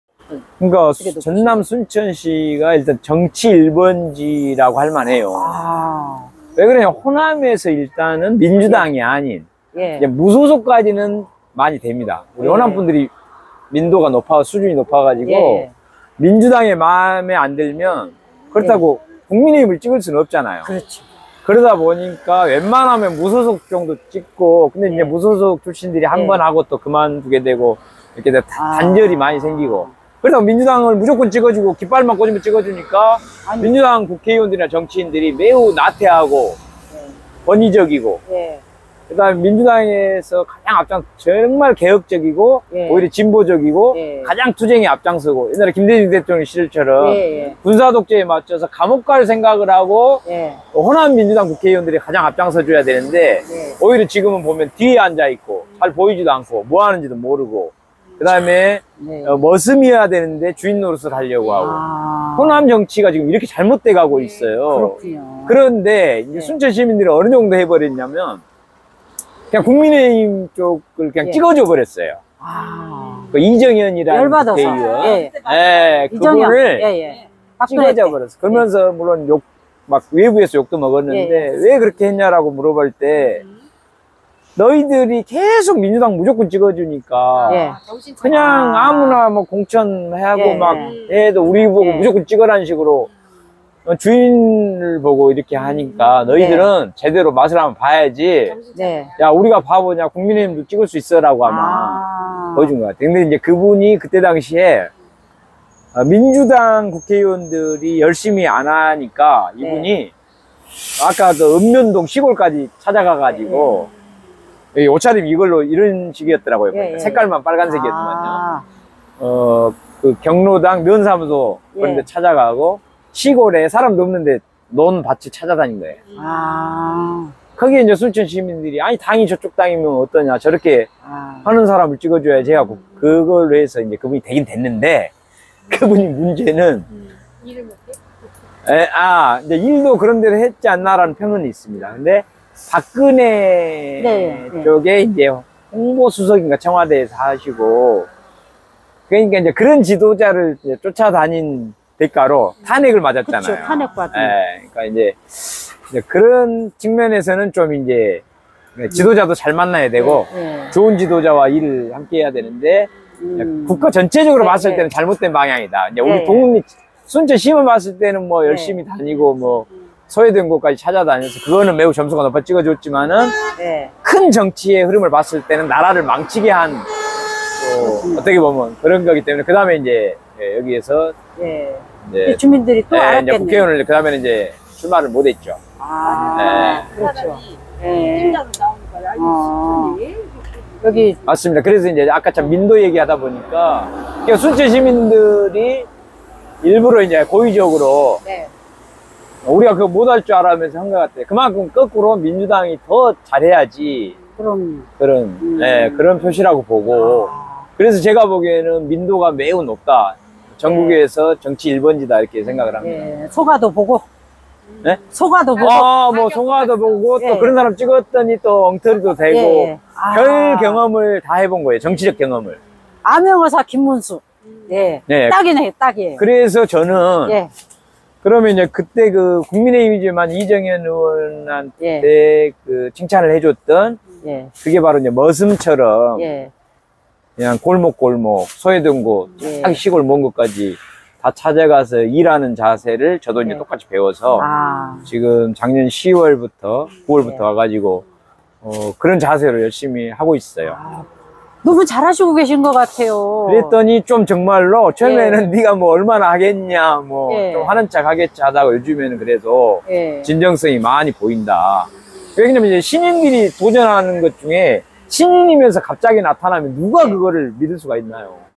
[230713] 정치1번지 순천! 암행어사 김문수가 골목골목 머슴처럼 뛰고 있어요, 진정성, 순천시민들이 반했어요~
내년
약력
전) 이재명대통령후보 비서실 선임팀장
전) 서울시의회 교육위원장
현) 더불어민주당 순천지역기본소득위원장
고려대정치외교학과졸업